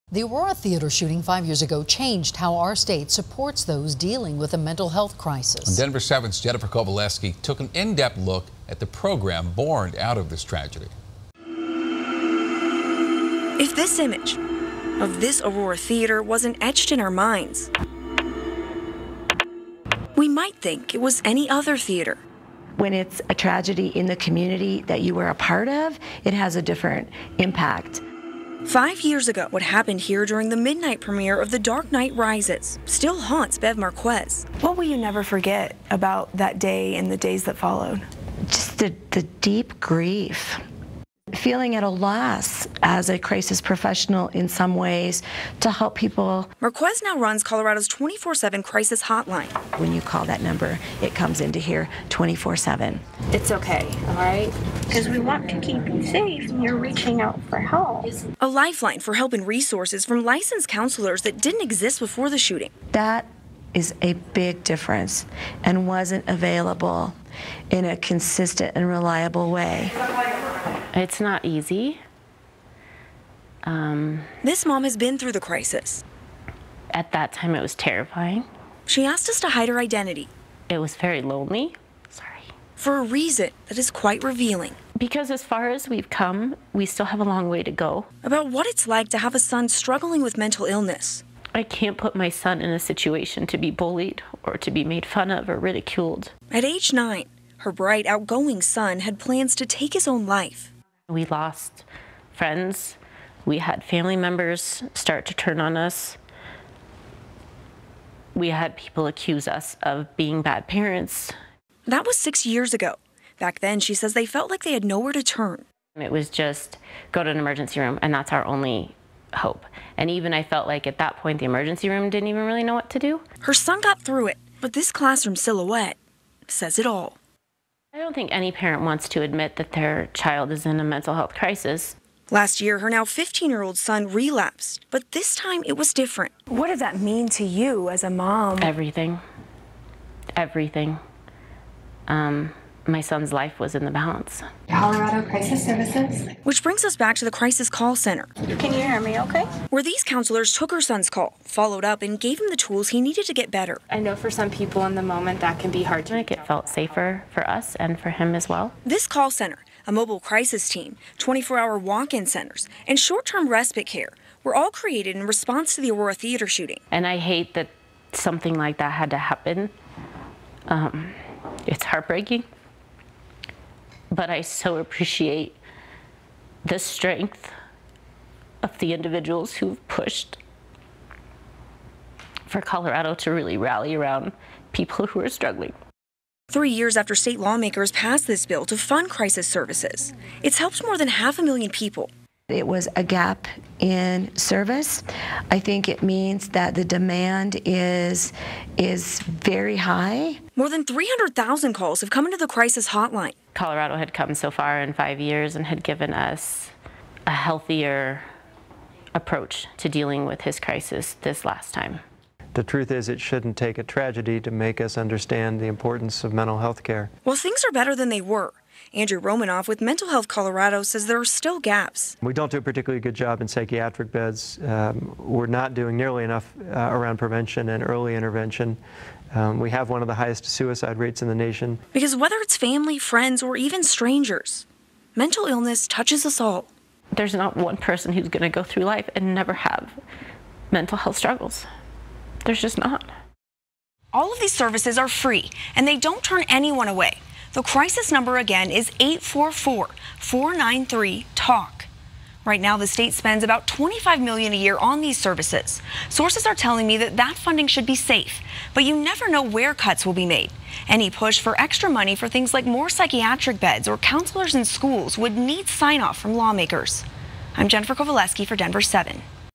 Five years after the Aurora theater shooting, what's the status of mental health care?
Five years later, Denver7 looks at what's working, who its helped and why the program could end soon.